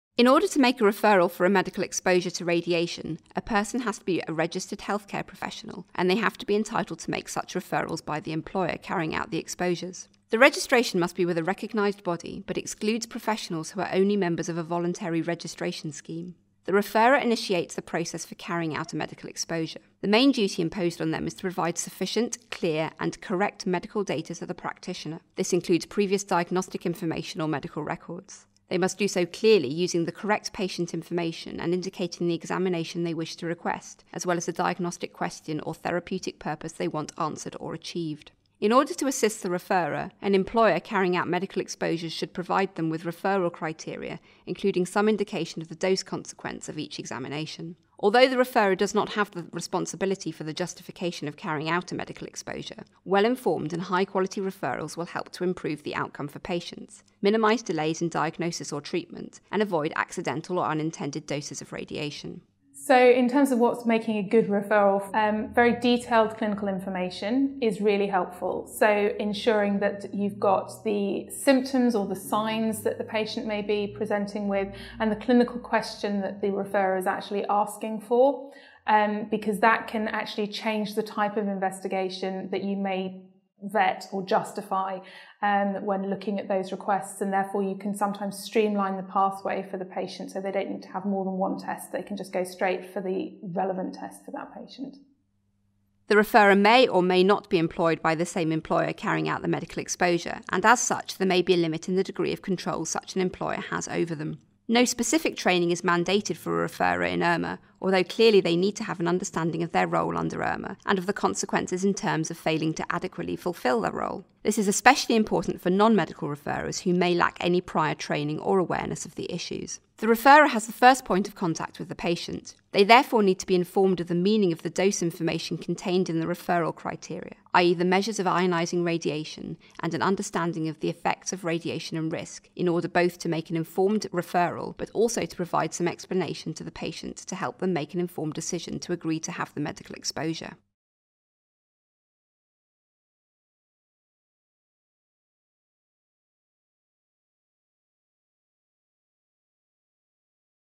BIR Video Course: Radiation Safety – Sample content
This course is for all members of your imaging team including radiographers, radiologists, physicists, managers and others.

This course provides 3 CPD credits in accordance with the CPD Scheme of The Royal College of Radiologists, and has been approved for professional accreditation by the College of Radiographers until March 2026.